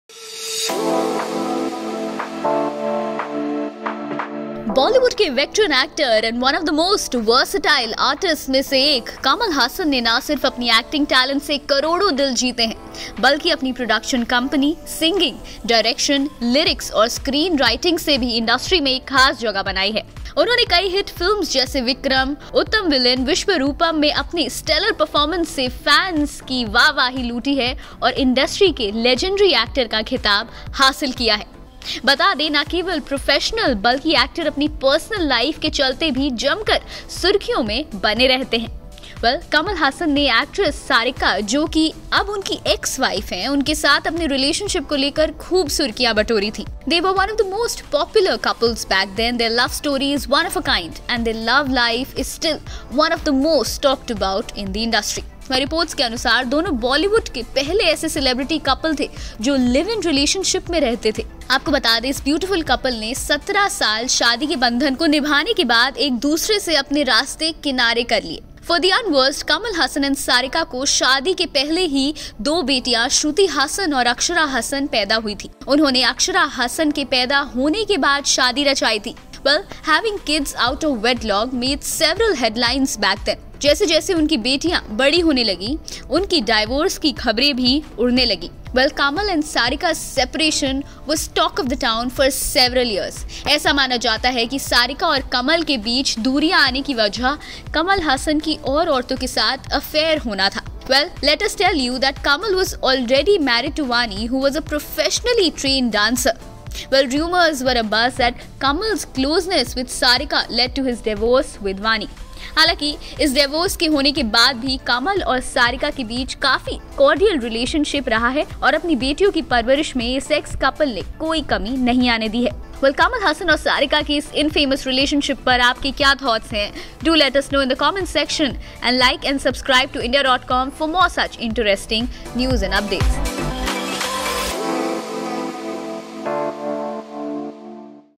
Kamal Haasan and Sarika's infamous love story | Bollywood affairs
Kamal Haasan and Sarika's love story was an infamous one but yet special. Their love grabbed a lot of eyeballs back then. Take a look at their complicated love story